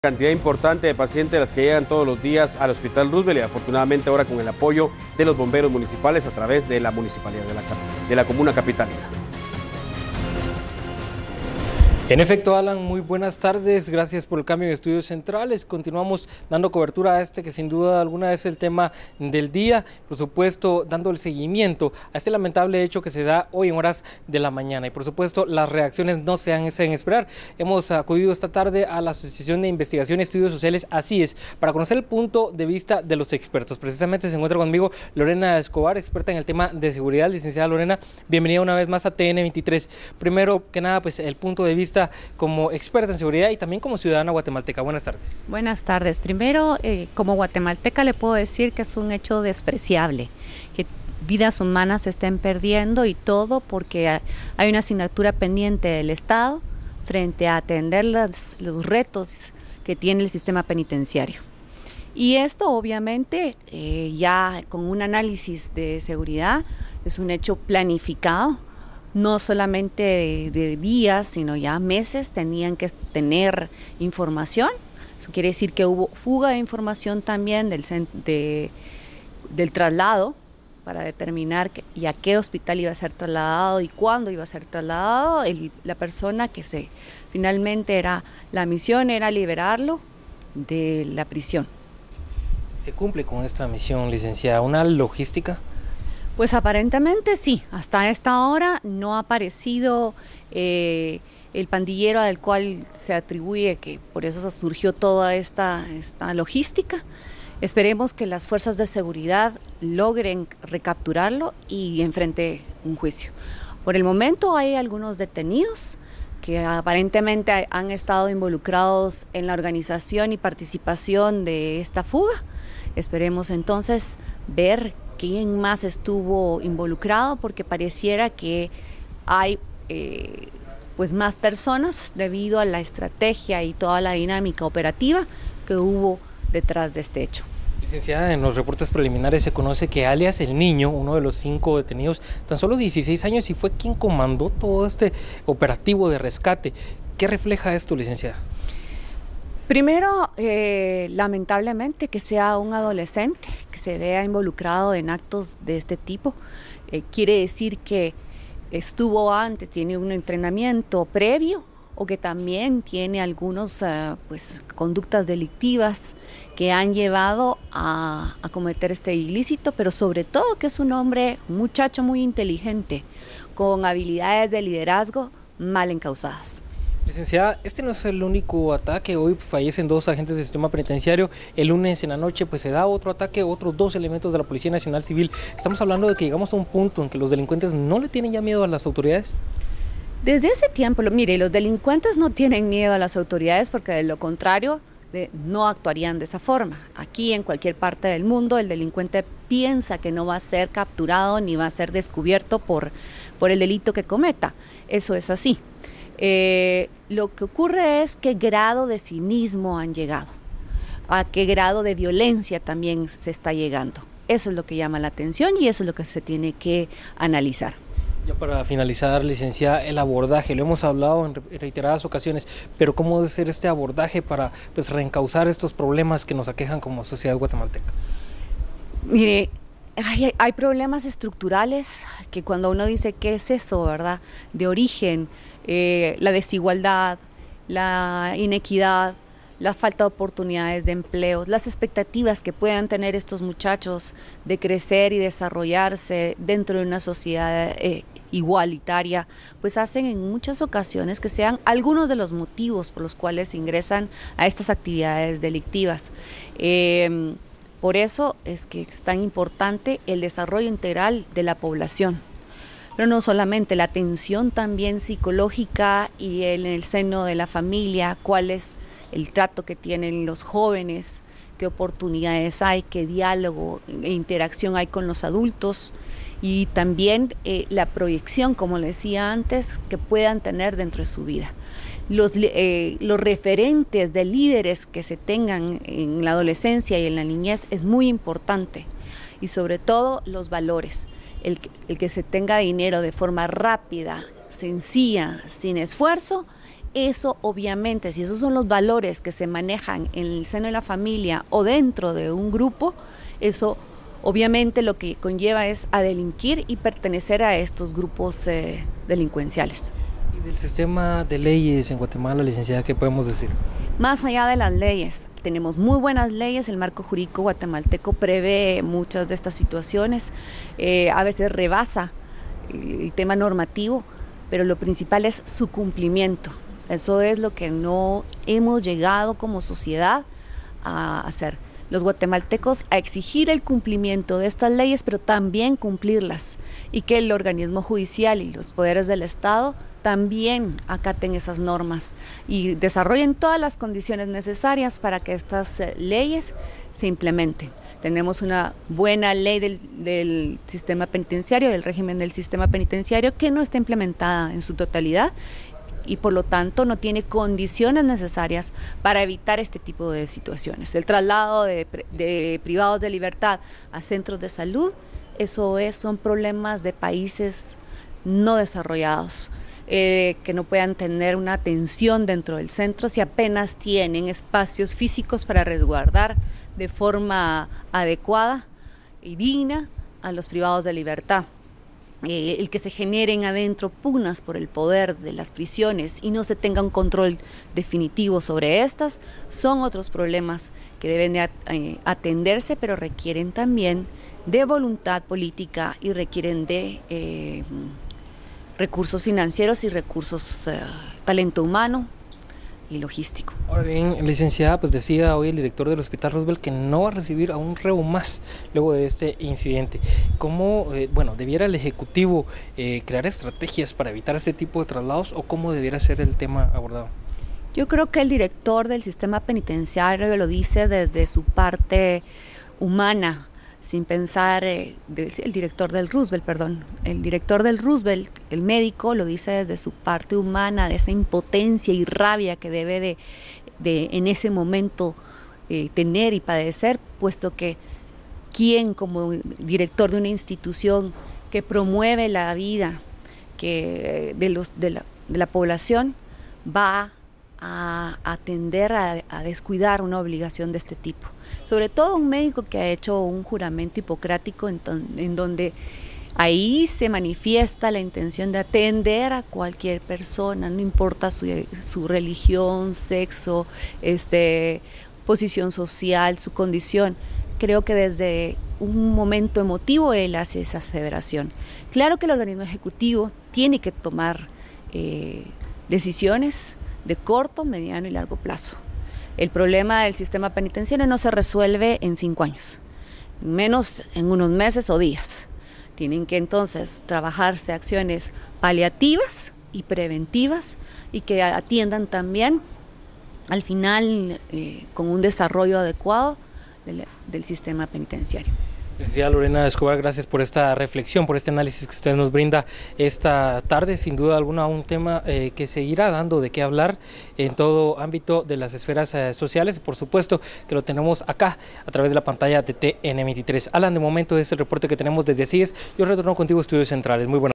Reacciones ante la masacre en el Hospital Roosevelt
Siete personas perdieron la vida en el atentado
Lorena Escobar, analiasta de Asíes nos brindó su punto de vista sobre la tragedia ocurrida en el área de emergencias del Hospital Roosevelt este miércoles.